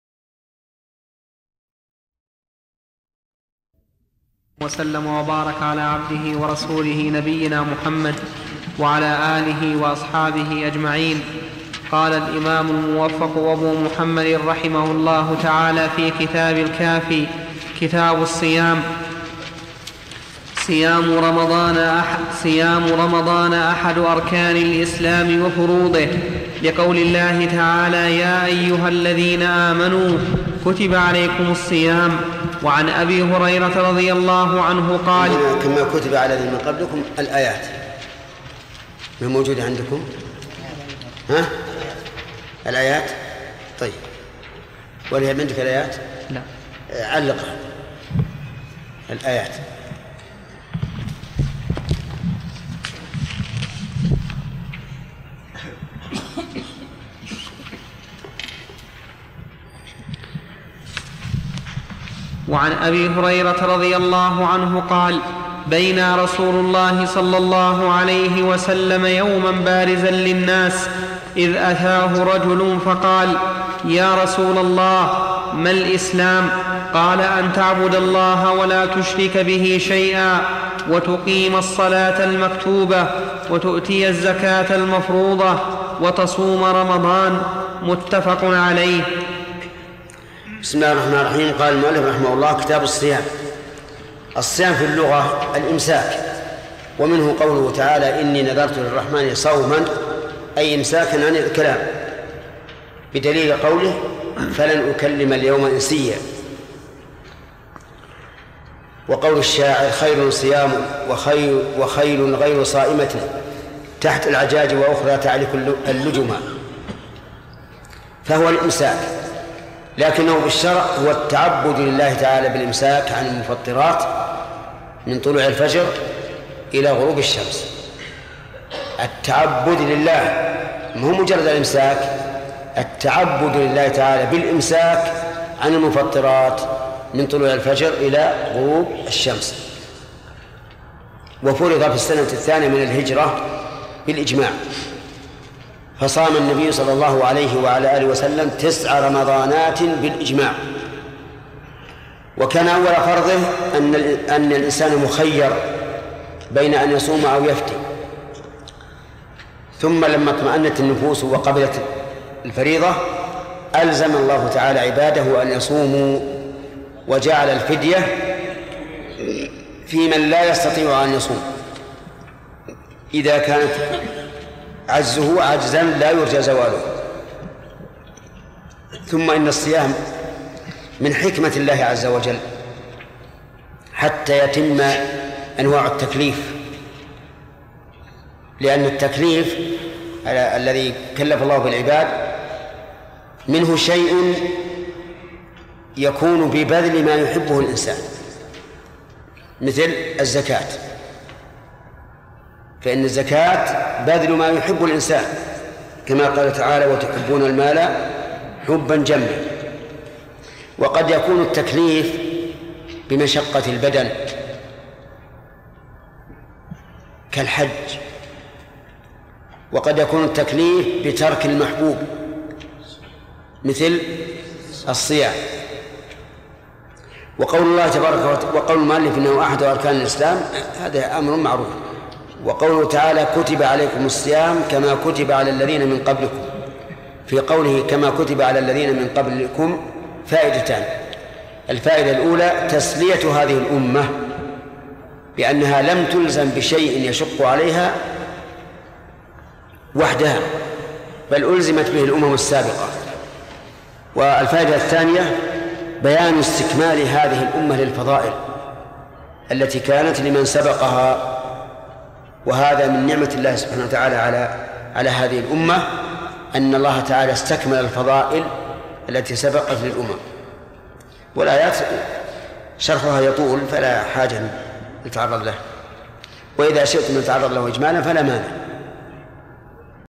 كتاب الصيام-الدرس الأول -الكافي في فقه الإمام أحمد بن حنبل - ابن عثيمين
كتاب الصيام-الدرس الأول -الكافي في فقه الإمام أحمد بن حنبل
القناة الرسمية : كن ايجابيا ||Be positive
شارك هذا الأجر 👈🏻 ليكون صدقة جارية عليك وعلى والديك 🤲🏻🌹